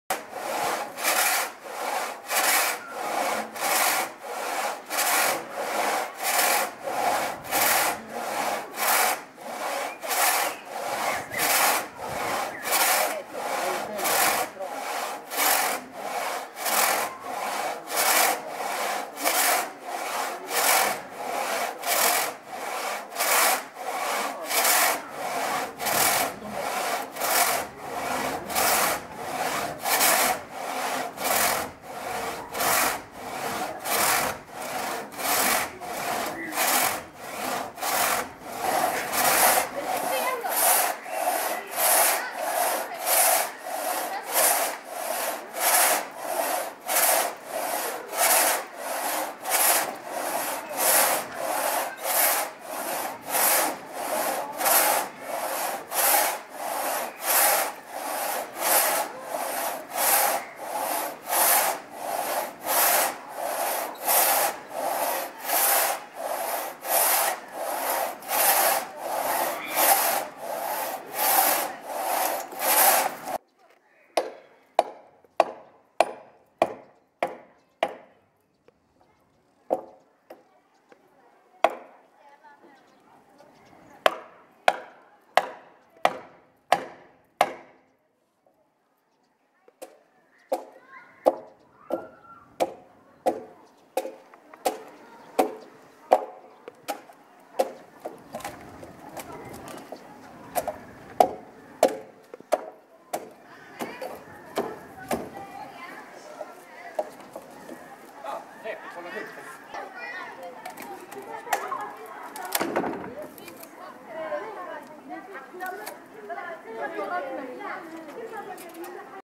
Kransågning
Kulturens timmermän Olof Andersson och Kristoffer Frederiksen kransågar bålar till Onsjöstugan. Läs mer på Kulturens blogg här